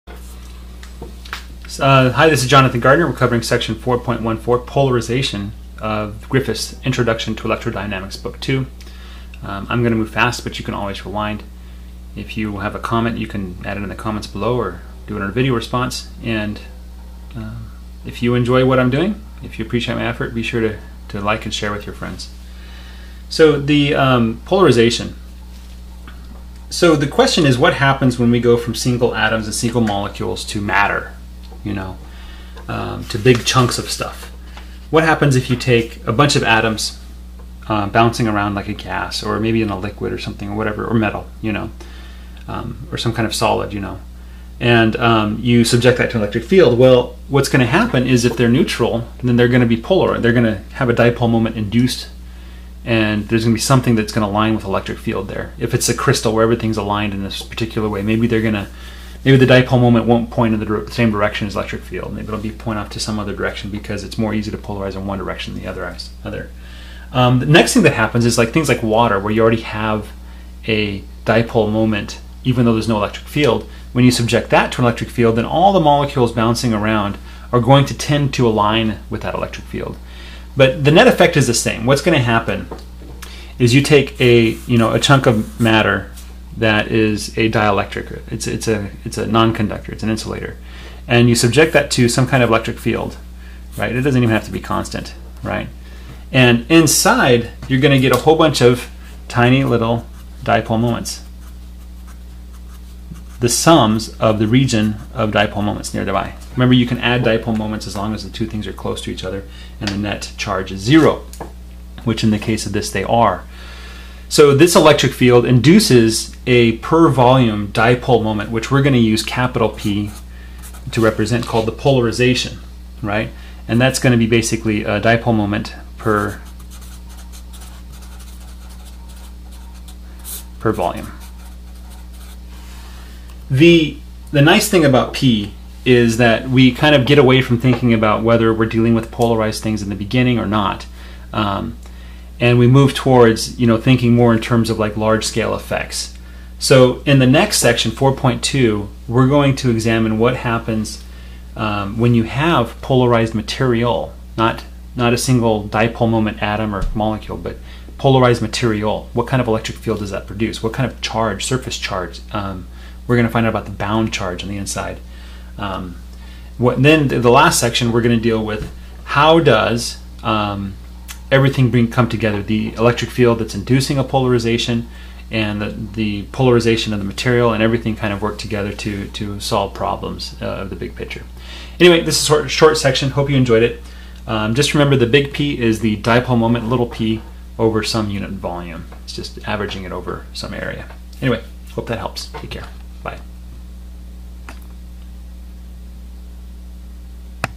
4.1.4 Polarization
The polarization of a dielectric is the total dipole moment in a given area divided by the volume of that area. It is a convenient way to summarize what has happened to the atoms and molecules within a substance, whether they were rotated, polarized, or pulled.